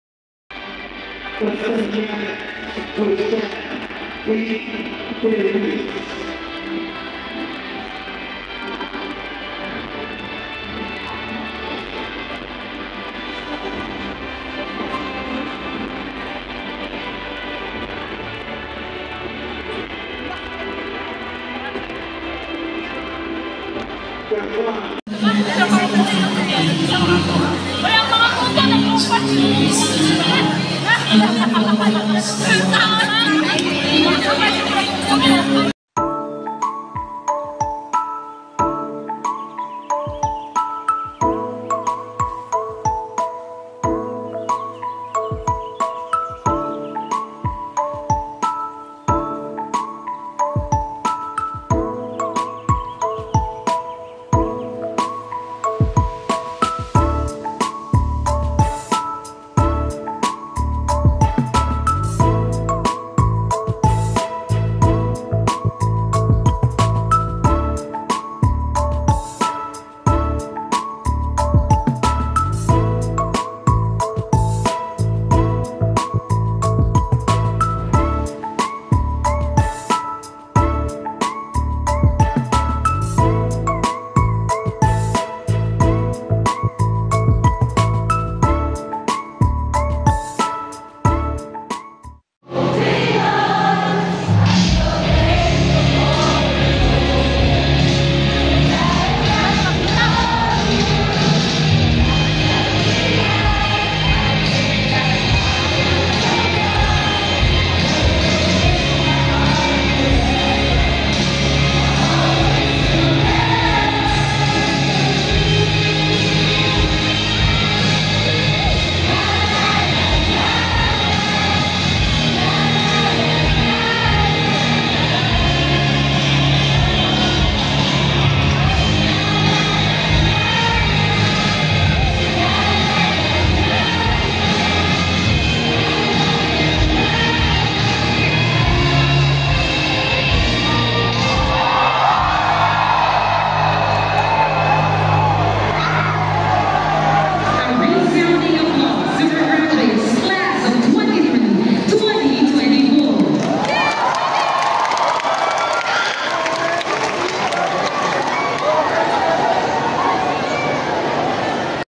Midwifery Student ➡️ Diploma in Midwifery
This vedios is my last event as a midwifery Student. 🧑‍🎓
I wis in my next journey I can survive and get that because that the only this I want it.
Be positive. ONE TAKE
Pray for my next journey.
DM ➡️ RM 🙏